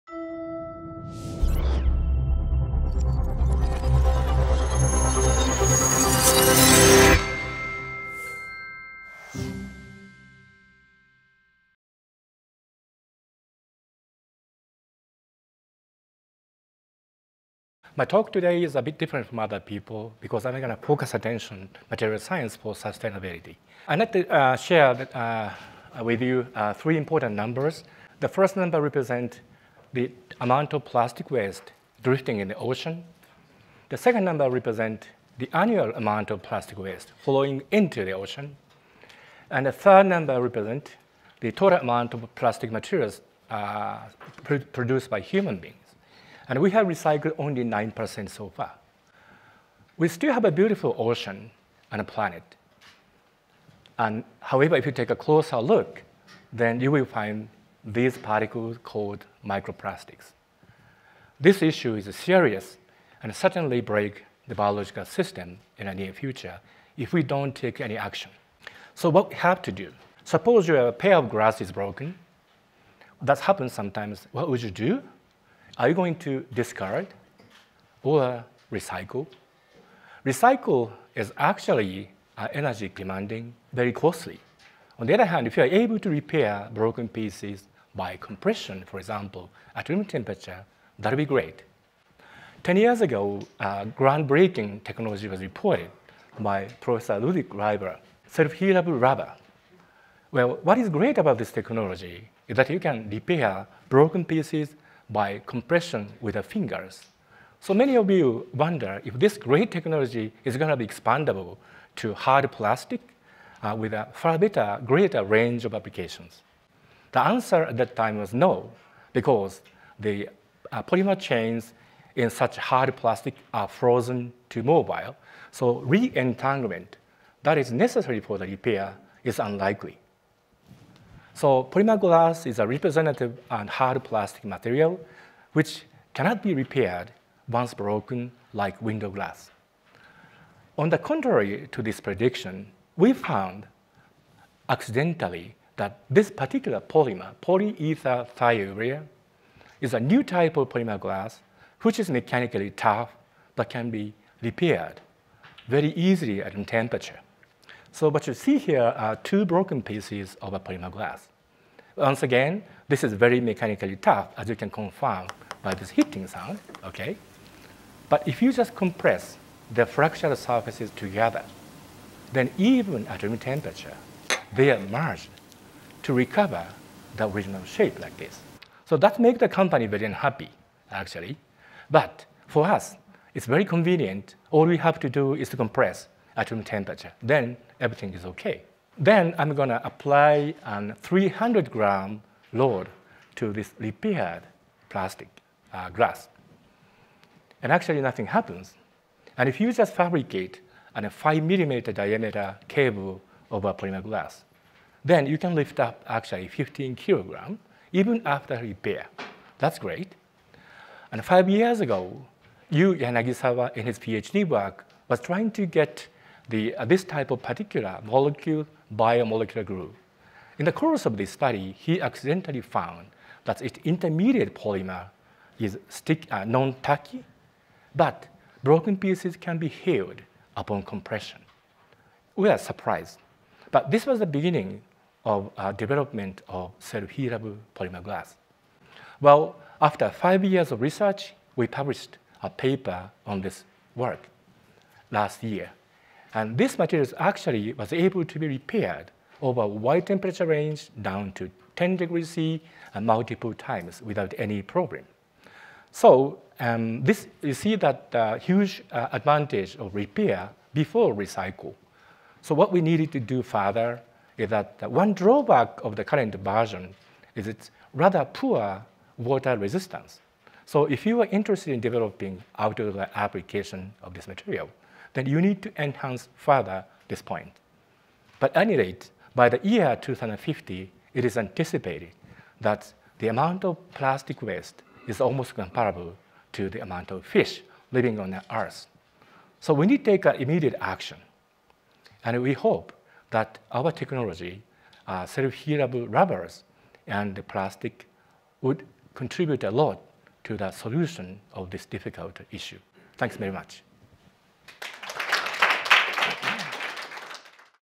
Hard, durable and self-healing materials | Takuzo Aida
Poly(ether thiourea) is a new kind of polymer glass which is tough but can be repaired simply by pressing together two broken pieces, even at room temperature. Takuzo Aida from the University of Tokyo explores the potential implications, such as reducing plastic waste.